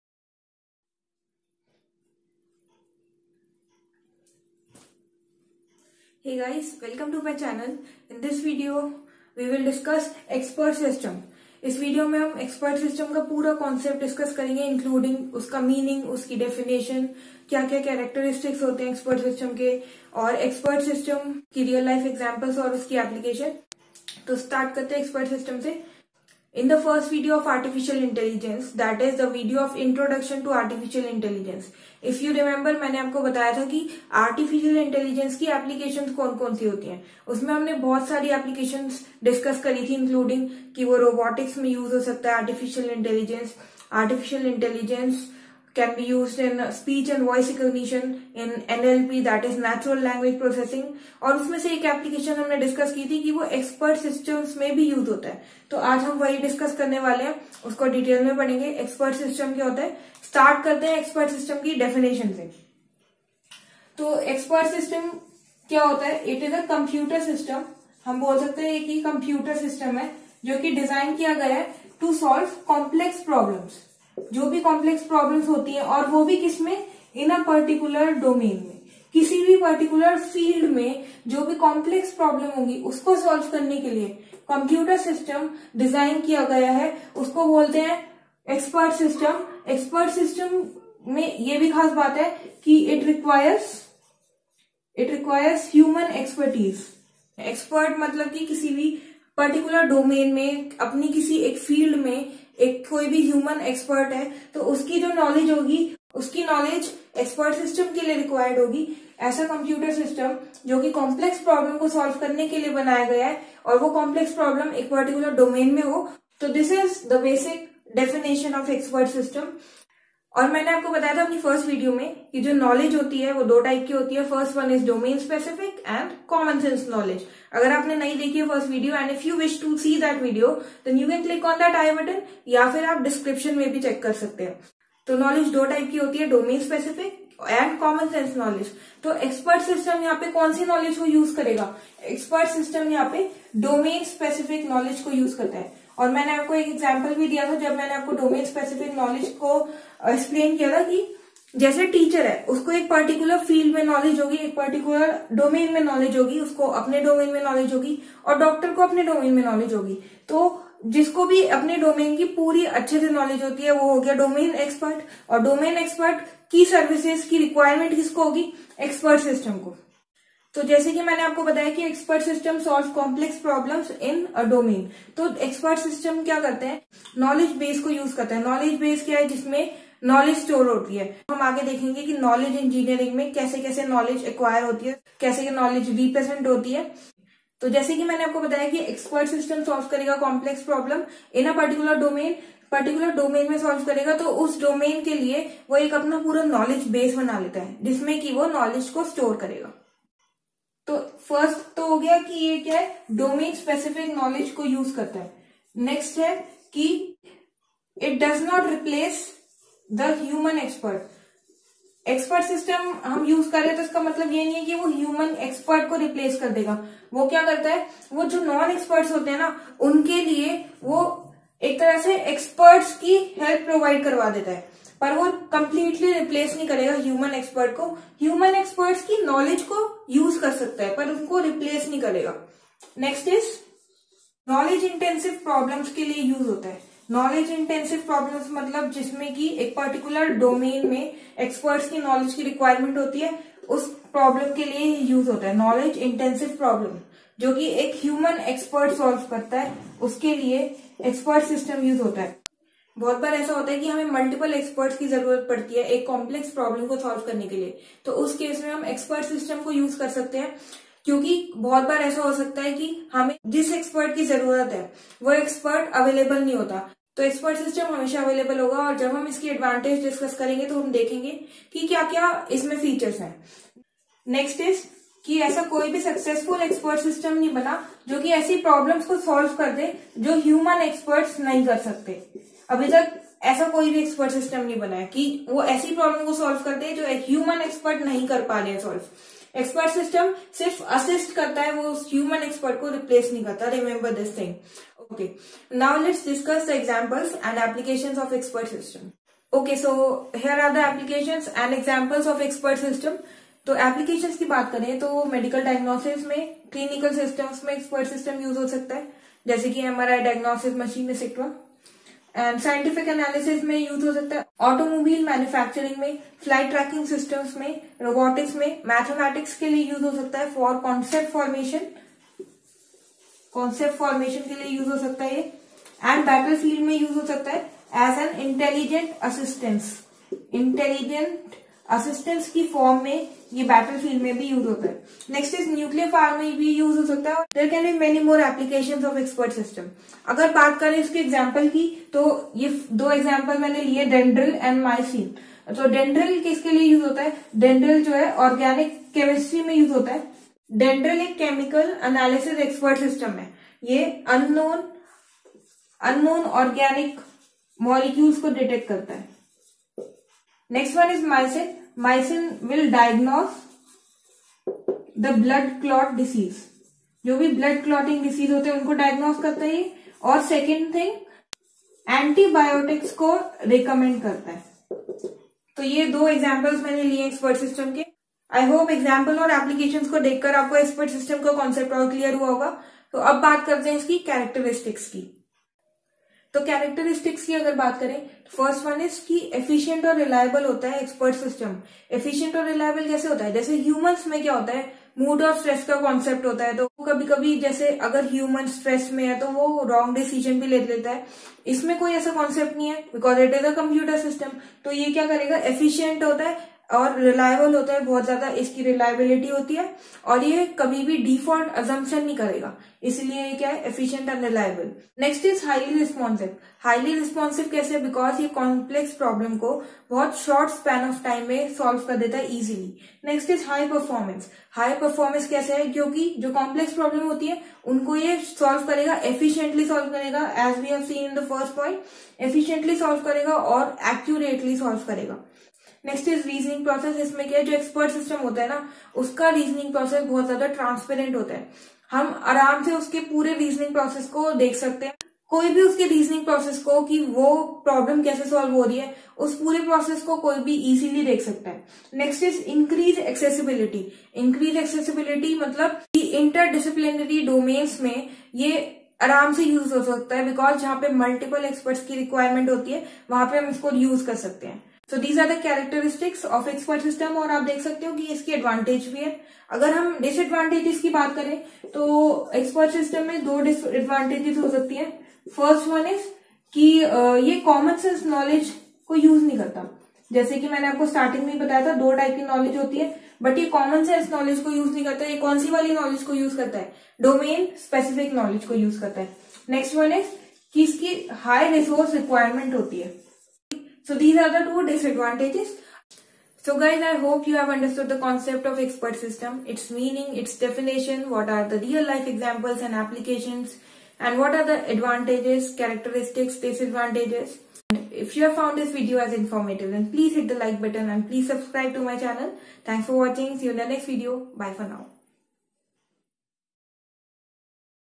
Expert System | Examples & Applications | Characteristics | Advantages & Disadv | AI - Kanika Vats
This video contains the explanation of EXPERT SYSTEM in Artificial Intelligence.

In this video the following topics have been covered :
• Meaning of Expert System
• Definition of Expert System
• Applications of Expert System
• Examples of Expert System
• Characteristics of Expert System
• Advantages and Disadvantages of Expert System